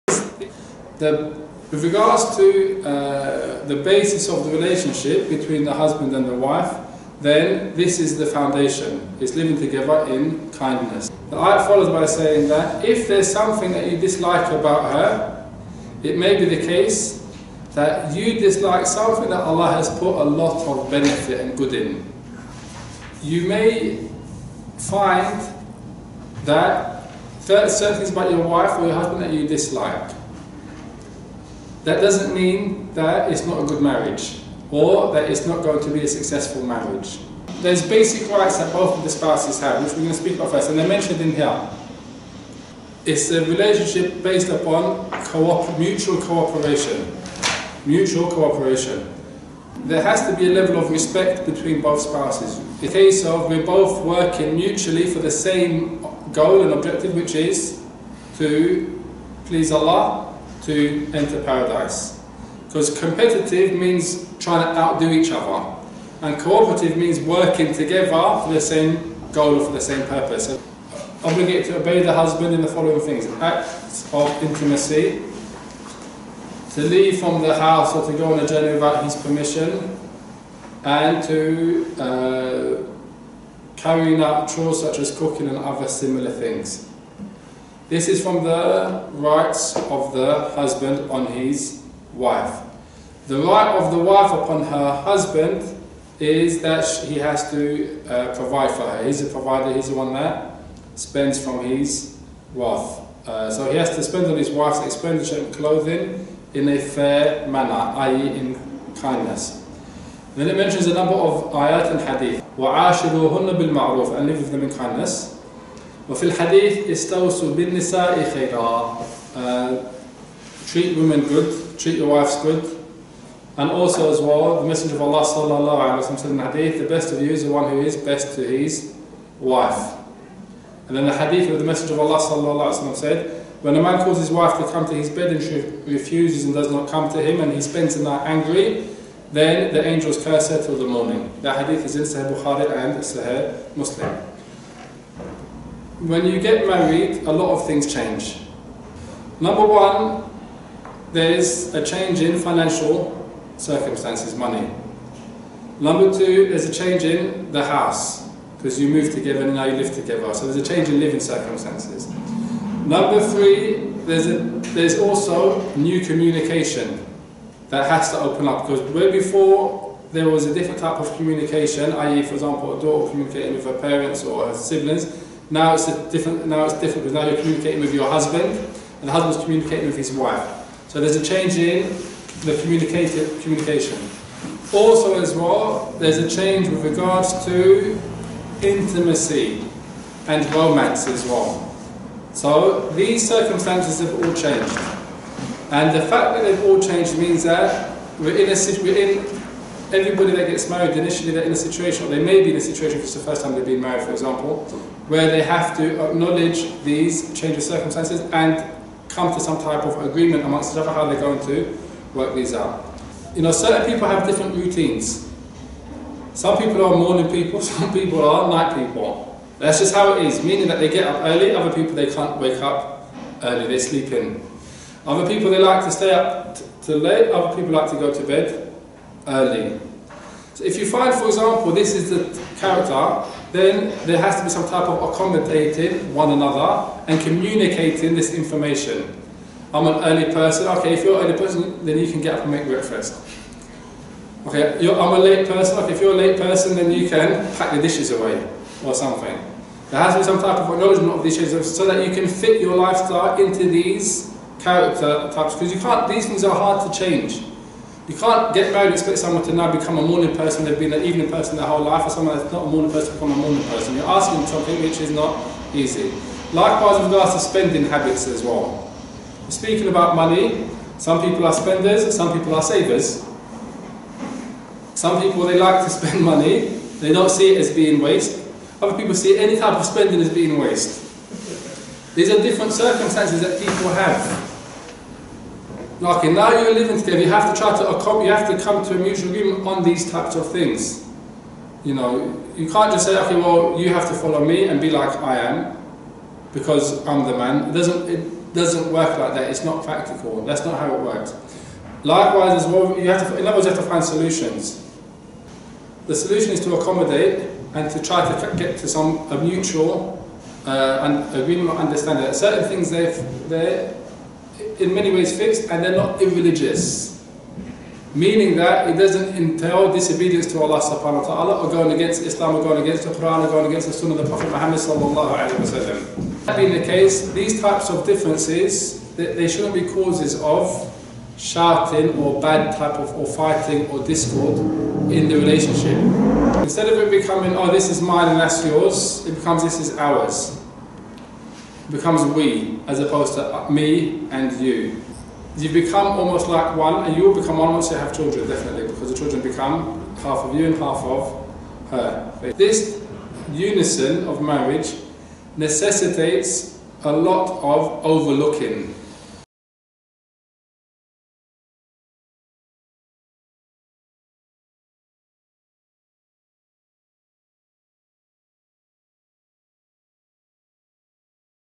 I'm married, now what?
What goes, what doesn't go and what else should I know. Selected clips from the LOve Inhale: Fiqh of Marriage One Day Course at Madinah College.